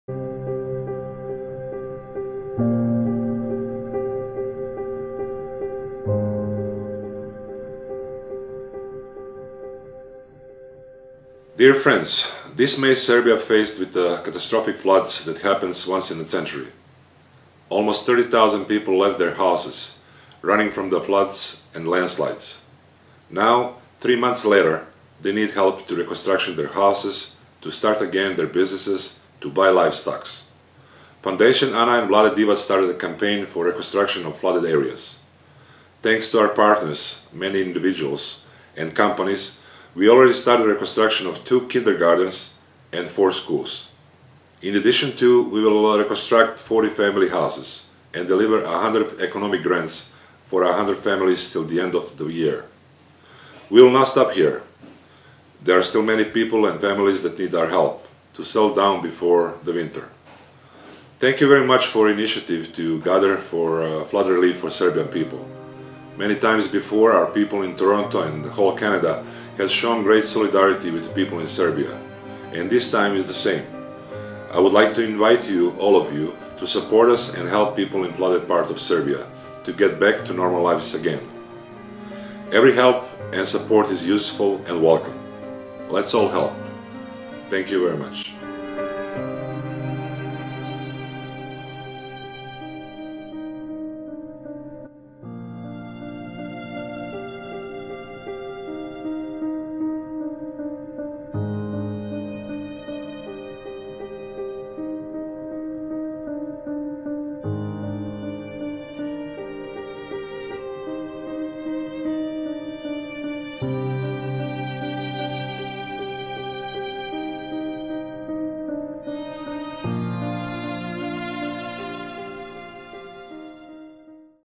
AUG 23 | SYP Building Hope Fundraising Gala & Auction | Ana & Vlade Divac Foundation
Building Hope Fundraising Gala & Auction | Serbian Young Professionals in support of the Ana & Vlade Divac Foundation | Saturday August 23, 2014 | Strachan Hall, Toronto.